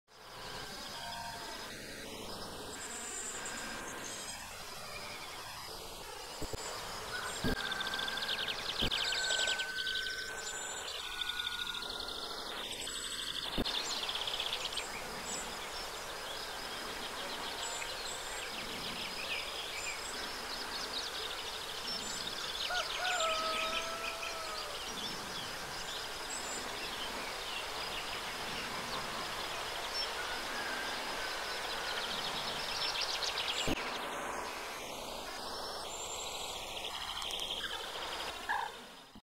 TOURTERELLE des Bois/Accouplement/Plus Tard ! BRUITX
Observations et Enregistrements dans une prairie humide de la Vilaine en amont du barrage d'Arzal dans le Morbihan : Le 13 Juin 2023 !